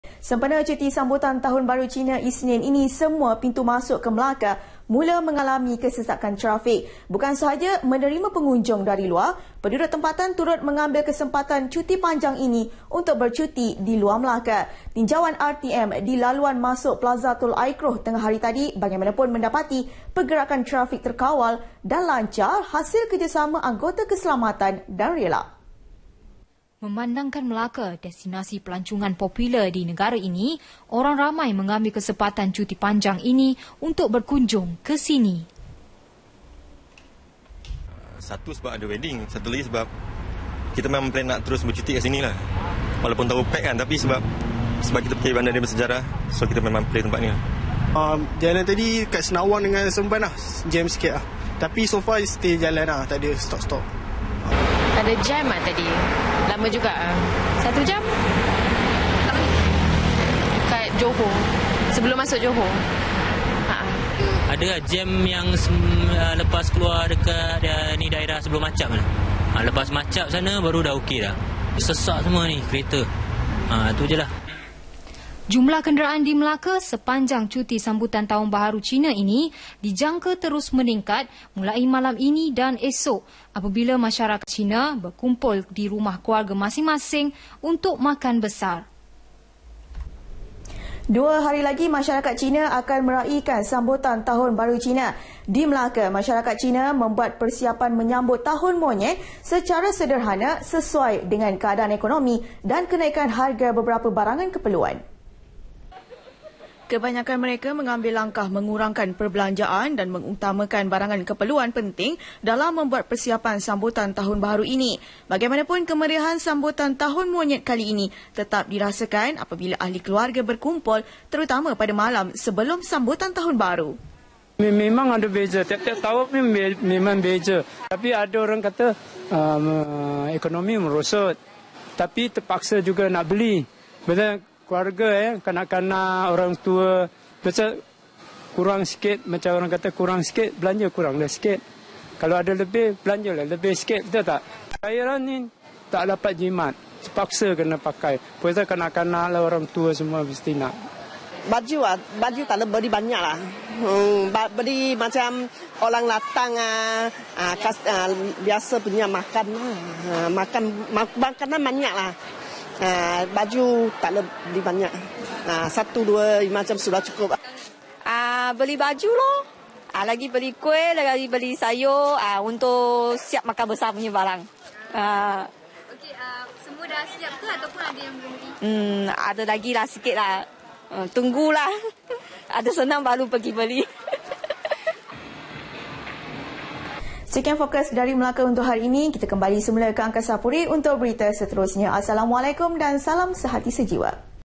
LANGSUNG DARI MELAKA 5 PM [6 FEB 2016]
1: TINJAUAN TRAFIK
Sempena cuti sambutan Tahun Baru Cina Isnin ini, semua pintu masuk ke Melaka mula mengalami kesesakan trafik
2: PERSIAPAN-TBC
Dua hari lagi, masyarakat Cina akan meraikan sambutan Tahun Baru Cina.
Di Melaka, masyarakat Cina membuat persiapan menyambut Tahun Monyet secara sederhana, sesuai dengan keadaan ekonomi dan kenaikan harga beberapa barangan keperluan.
3: MELAKA UNITED
Pasukan bola sepak Melaka. Melaka United kini hanya menunggu sepak mula Liga Perdana 2016, 12 Februari ini.